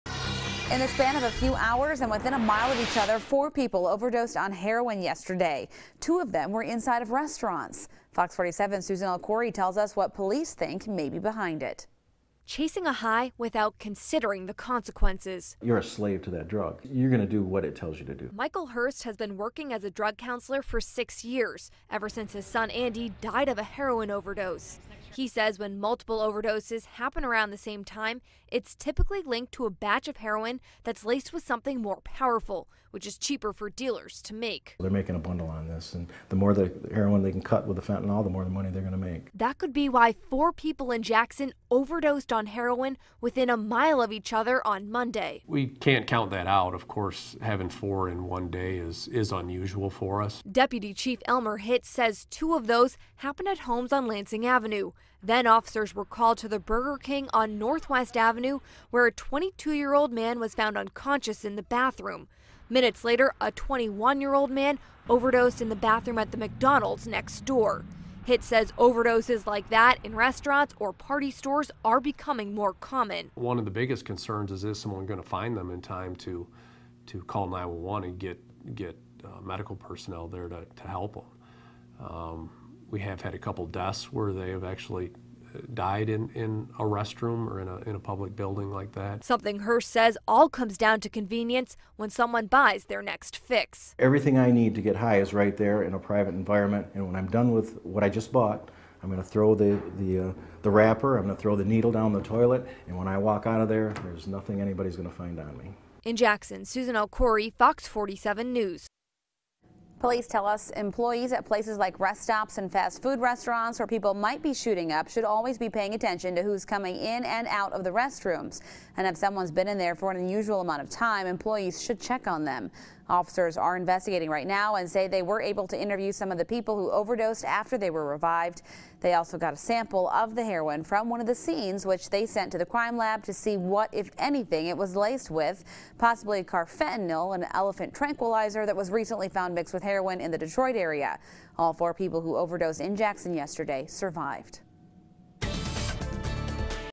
Four overdoses within a mile in Jackson
In the span of a few hours and within a mile of each other, four people overdosed on heroin in Jackson on Tuesday. Two of them were inside restaurants. FOX 47's Susan El-Khoury tells us what police think may be behind it.